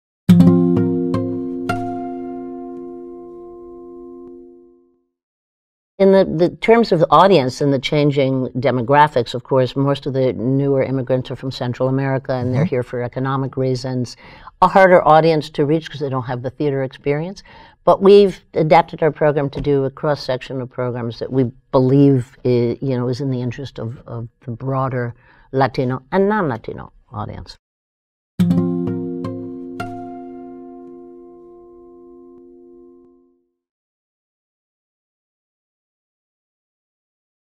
Preview: GALA Hispanic Theatre - Rebecca Medrano
Rebecca Read Medrano, Co-founder and Executive Director of GALA Hispanic Theatre, discusses how the theater's programs speak to a wide array of Latin American cultures and values.  This interview was produced in collaboration with WHUT (Howard University Television).